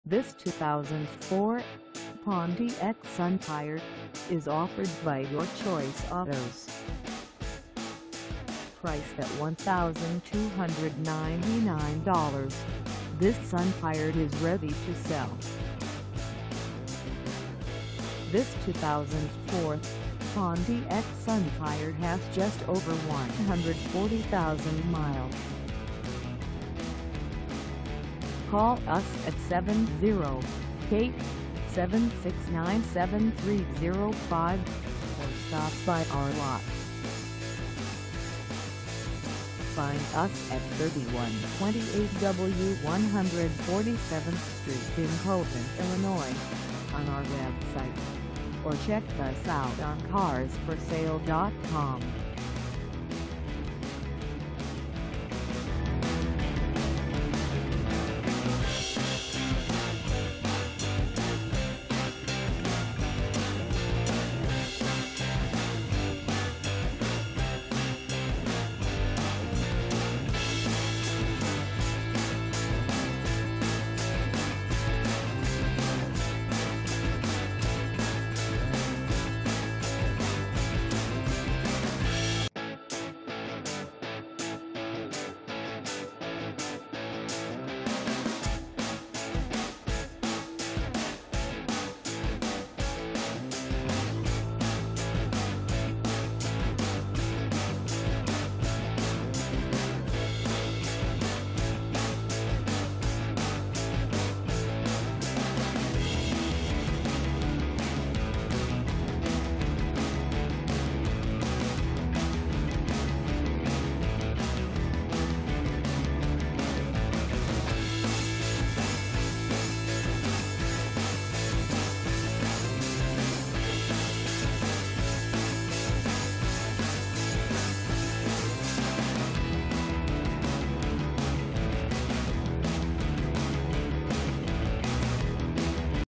2004 Pontiac Sunfire 2dr Coupe for sale in Posen, IL 60469 a
This 2004 Pontiac Sunfire 2dr Coupe is for sale in Posen, IL 60469 at Your Choice Autos .

Engine: 2.2L I4
Transmission: Automatic
Color: Red
Mileage: 140000
Contact Your Choice Autos 3128 W. 147th St Posen, IL 60469 to test drive this 2004 Pontiac Sunfire 2dr Coupe today.